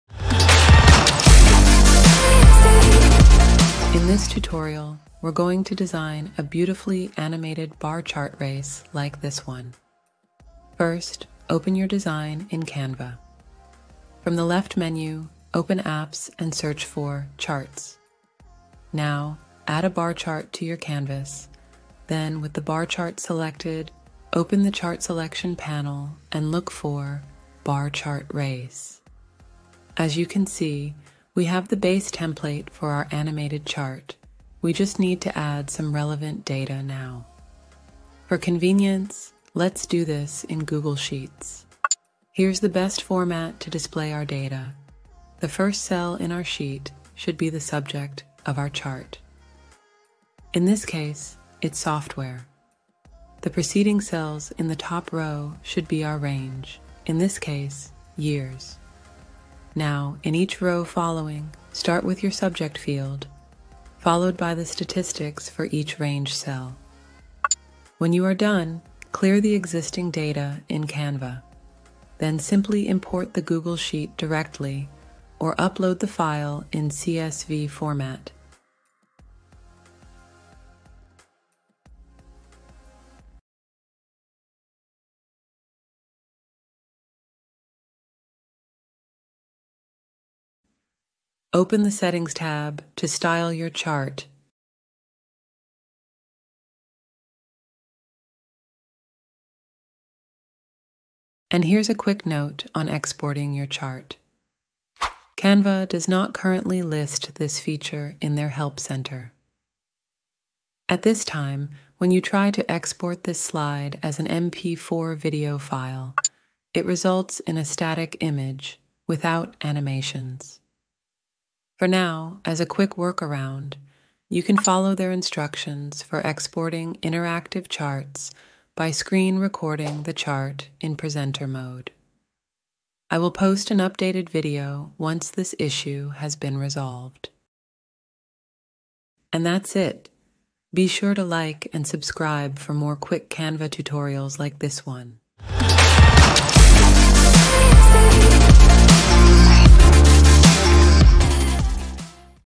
Create an animated BAR CHART RACE in Canva
Today, we're taking data visualization to the next level. I'll guide you through the exciting process of crafting your own animated bar chart race right in Canva. 📊

🚀 Video Overview 🚀

In this tutorial, you'll learn:

How to create captivating animated bar chart races in Canva
Steps to design stunning data visualizations
Tips and tricks for effective data animation
📈 Outro 📈

Thank you for joining us in this exciting data animation journey. With your newfound skills, you can bring data to life and tell compelling stories through animated bar chart races. Keep experimenting, keep learning, and most importantly, keep designing! Until next time, happy charting!